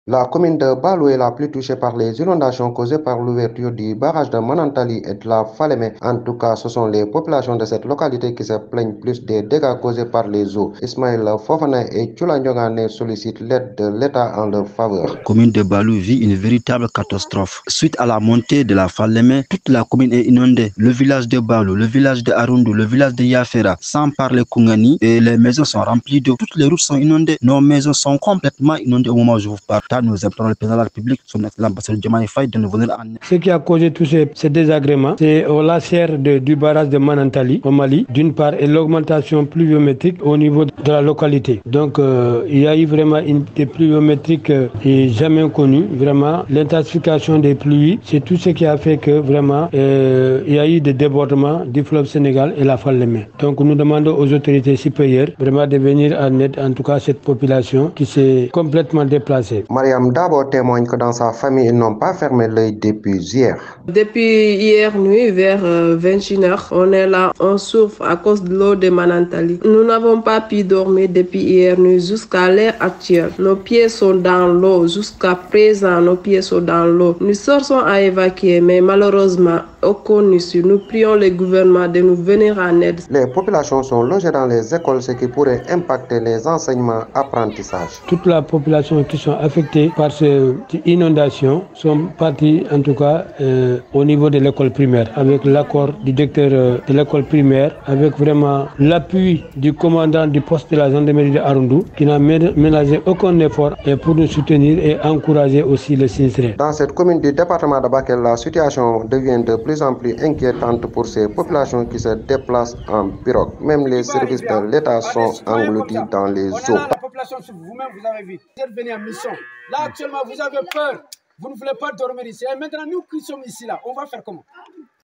Urgence à Ballou : les populations submergées implorent des secours
Le trop-plein de la Falemé, provoqué par l’ouverture du barrage de Manantali, a plongé Ballou sous les eaux. Les sinistrés, en détresse, appellent les autorités et les partenaires à leur venir en aide.